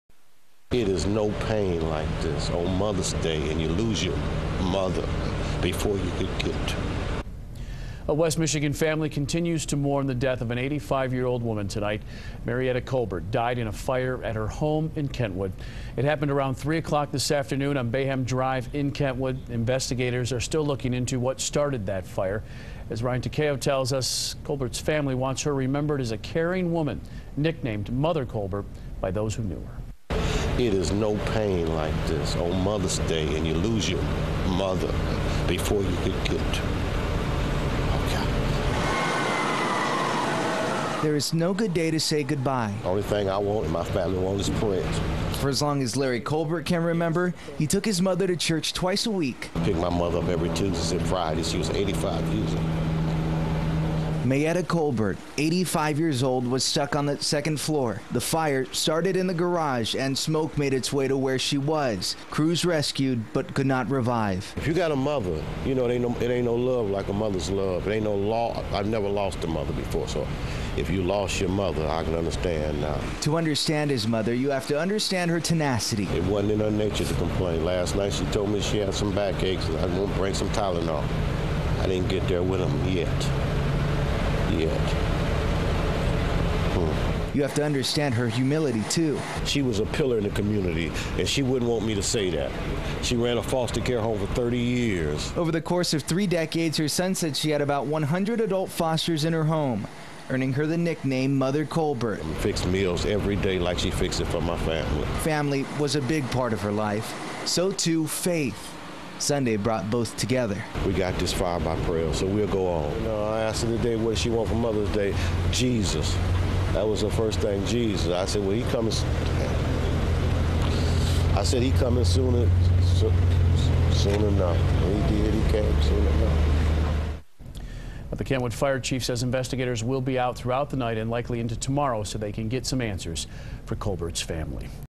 Grandma, adult fosterer dead in fire
Mayetta "Mother" Cobert ran an adult foster care home.